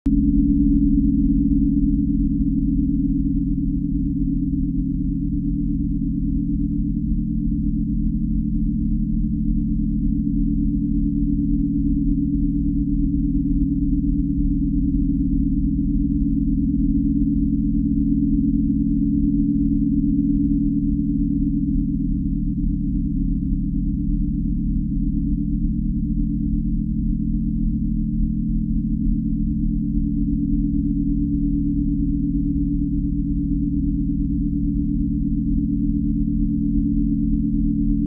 carbon
Sonifikacja linii spektralnych atomu węgla C I.

Sonification of Carbon C I spectral lines.